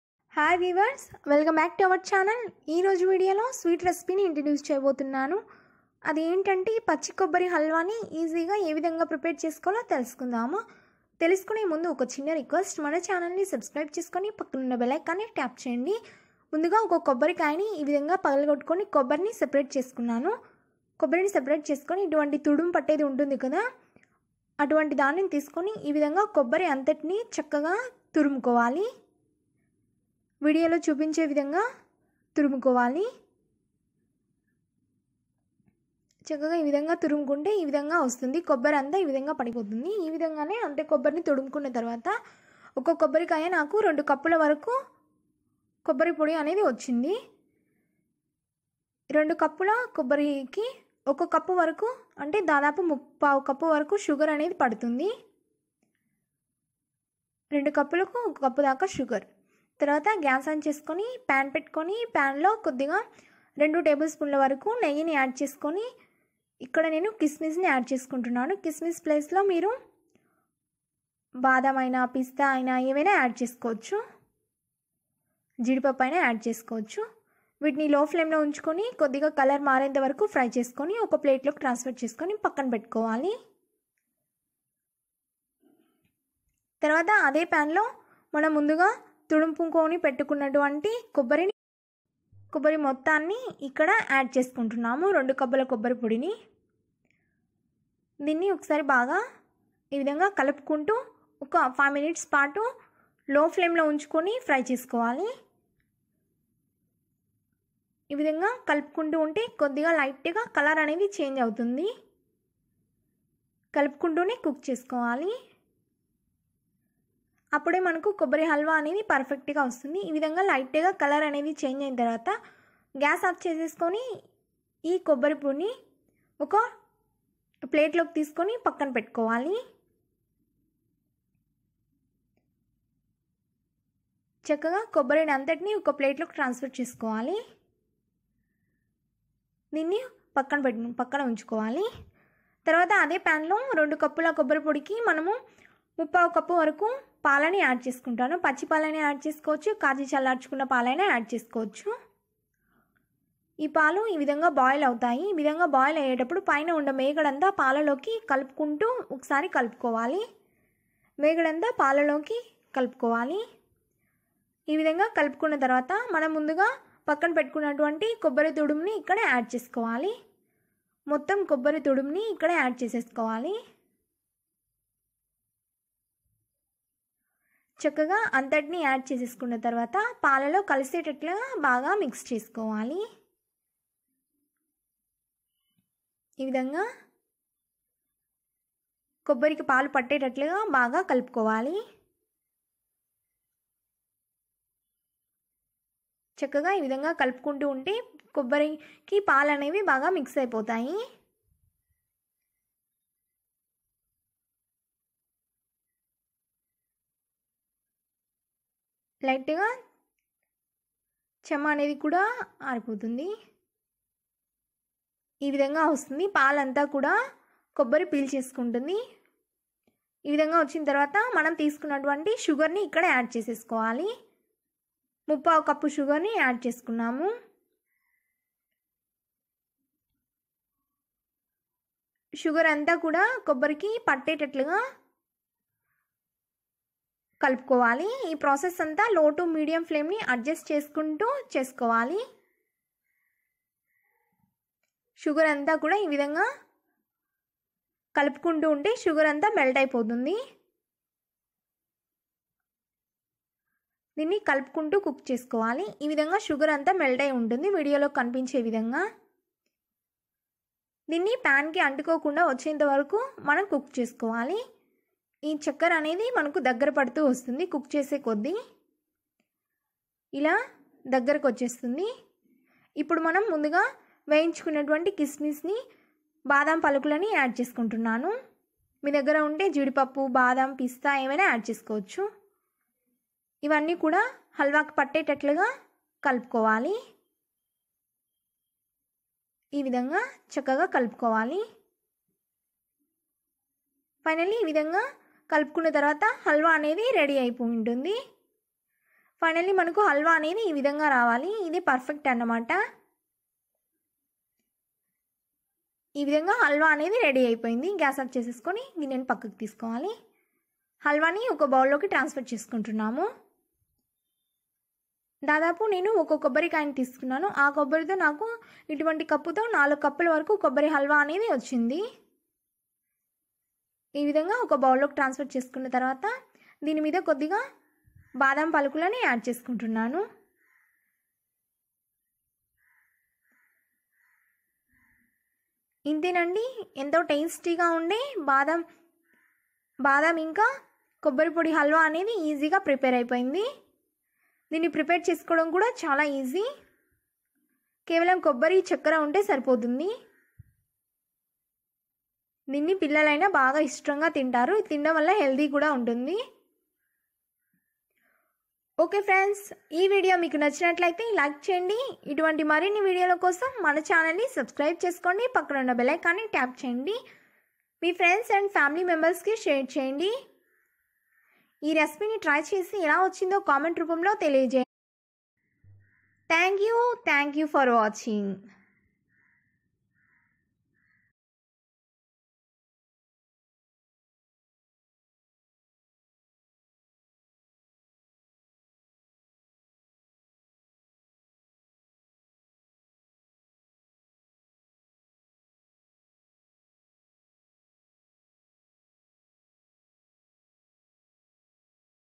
Coconut Halwa || కొబ్బరి హల్వా || ఆరోగ్యకరమైన హల్వా ని ఇలా ఒకసారి ట్రై చేయండి
మరిన్ని వీడియోల‌ కోసం మన చానెల్ ను సబ్స్క్రయిబ్ చేసుకోండి పక్కనున్న బెల్ ను నొక్కండి.

Ingredients

Coconut powder 2 cups
Kismis  5
Badampappu 8